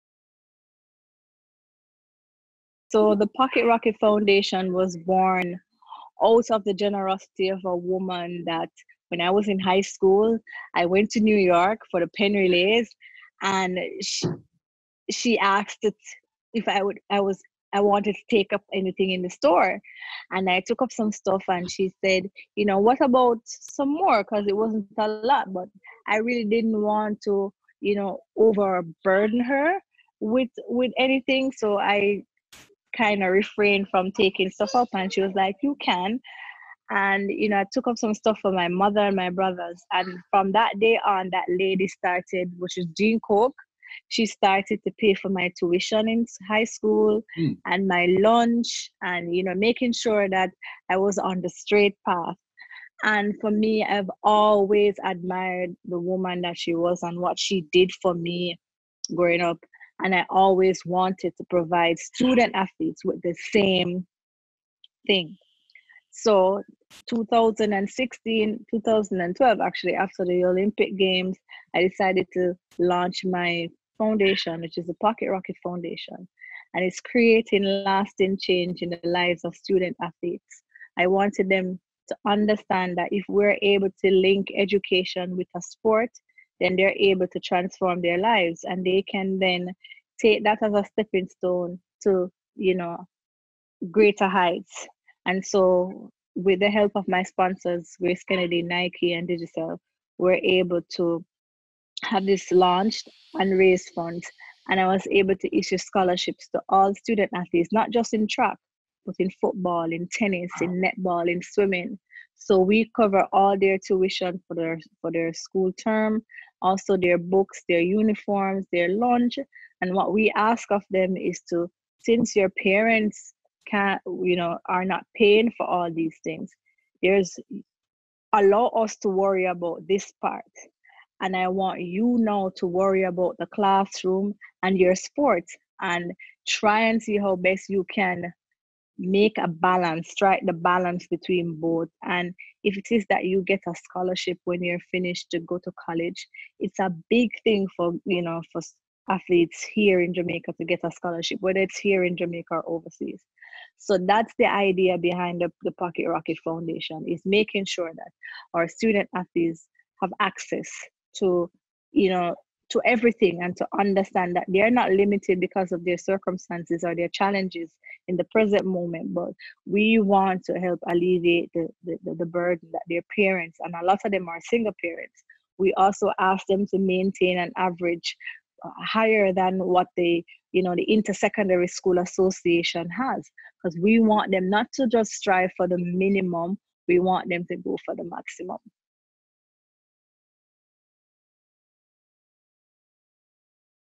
Q&A Extras: Shelly Ann Fraser Pryce
Shelly Ann Fraser Pryce, the greatest female sprinter of all time, speaks passionately about her Pocket Rocket Foundation, which supports student athletes in Jamaica.